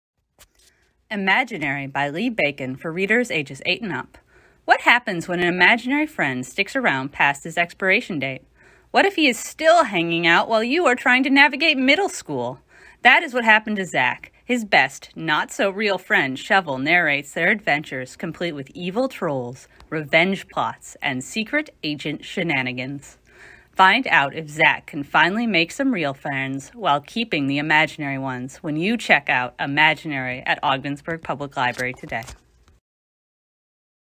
Book Talk: "Imaginary" by Lee Bacon
"Imaginary" by Lee Bacon is this week's book recommendation.